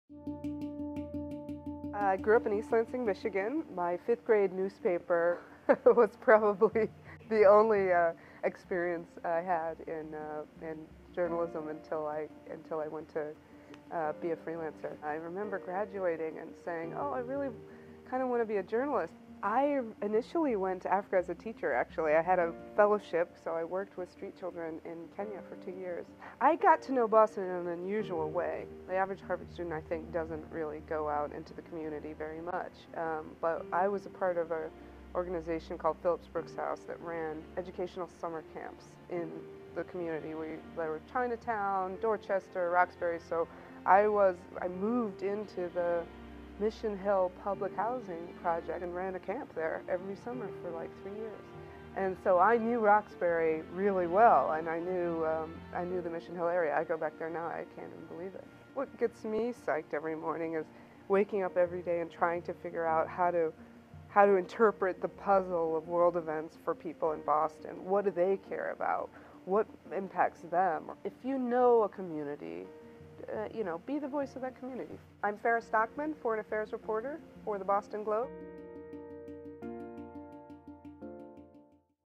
The Boston Globe Journalist Series: Farah Stockman
An interview with Farah Stockman, a Boston Globe Editorial Reporter.